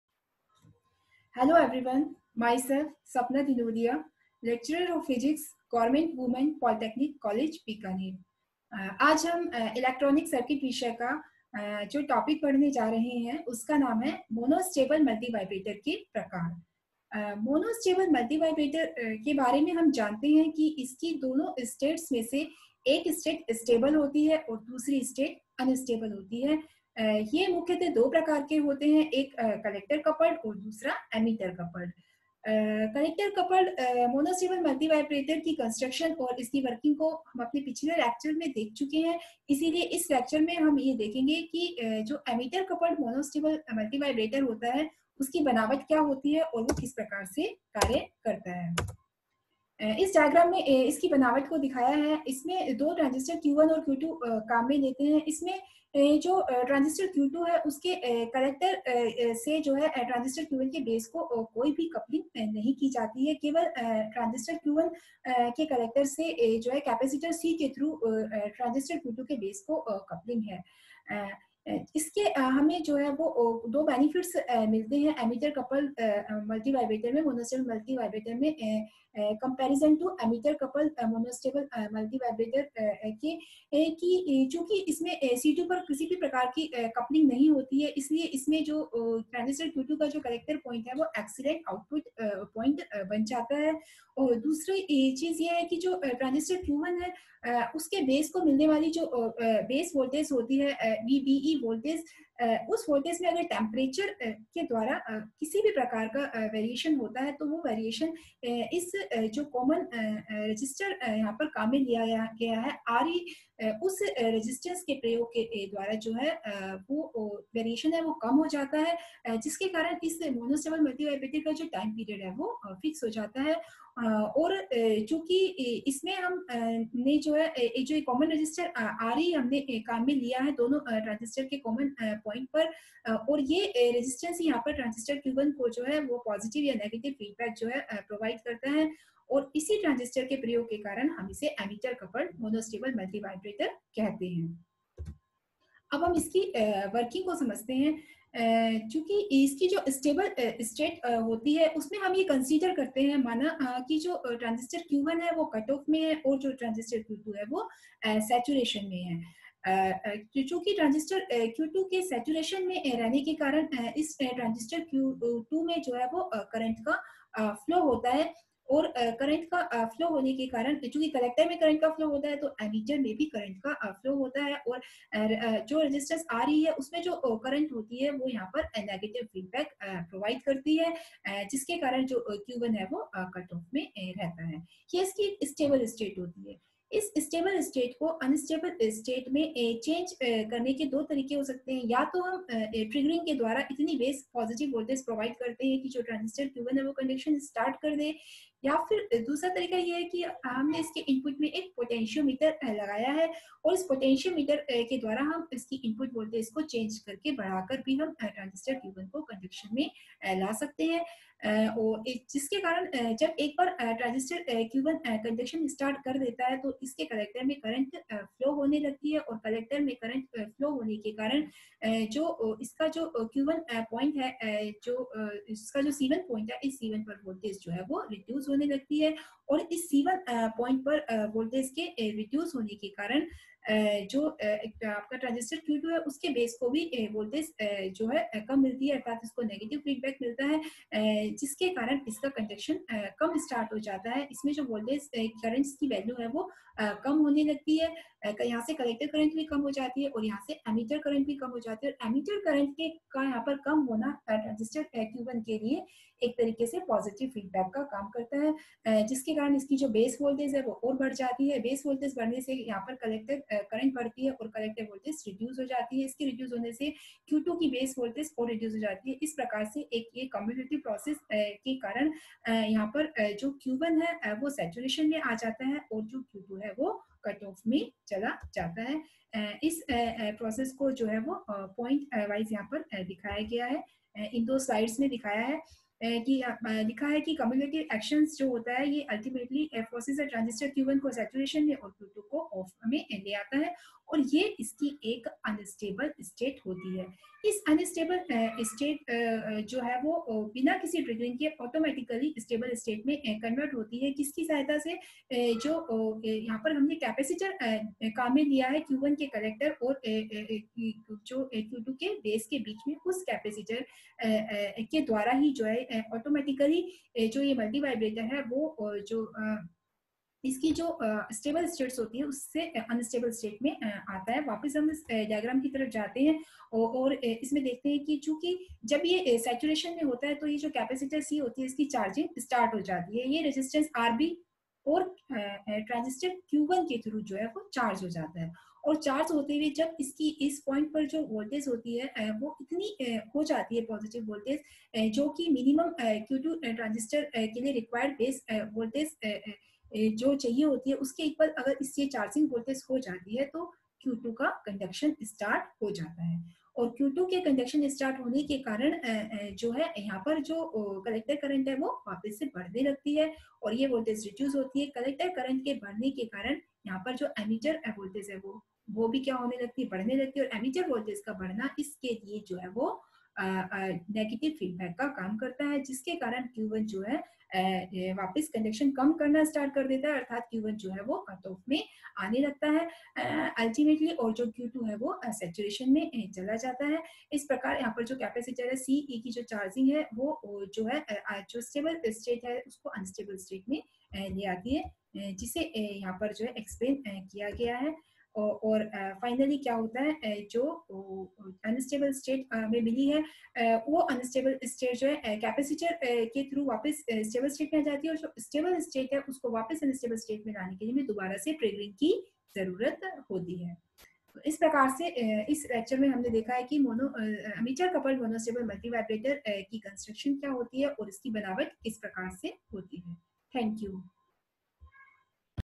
Emitter coupled monostable multivibrator EL 301 Sapna Dinodiya Lecturer GWPC Bikaner
This video is in accordance with the syllabus of the Polytechnic College of Rajasthan prescribed by BTER, Unit 7 EL301 Electronic circuits